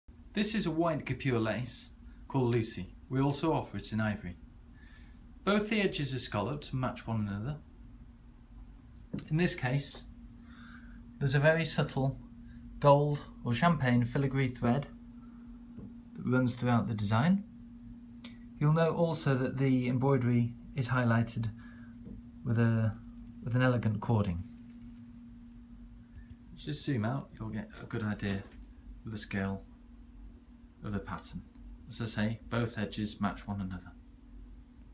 White Guipure Lace - Lucy
A stunning white guipure lace fabric with a champagne thread adding to its beauty